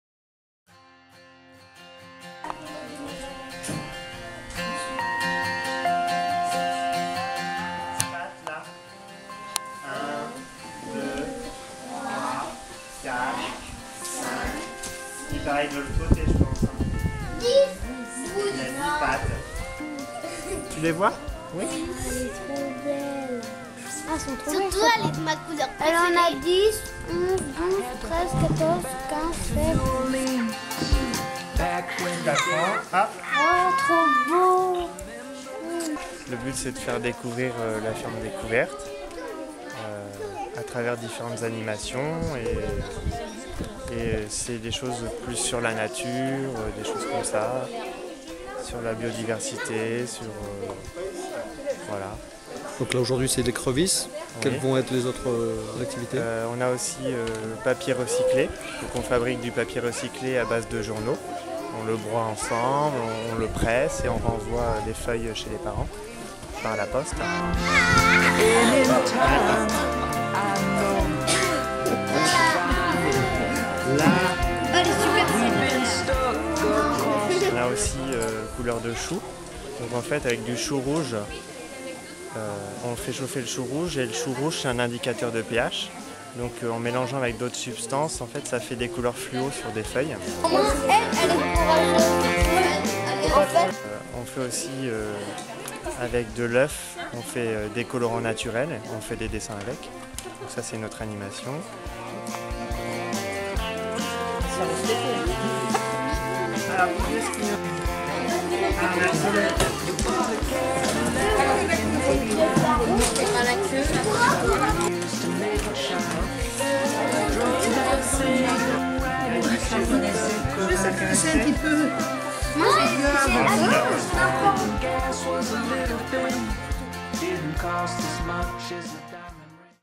VIDEO Thuré: des écrevisses à la rencontre des enfants
Tout l'été, la ferme-découverte de Thuré organise des ateliers buisssonniers le jeudi après-midi à destination des enfants. Réalisation de papiers recyclés, peinture à l'oeuf ou découverte de l'écrevisse, les vacances sont ludiques et utiles.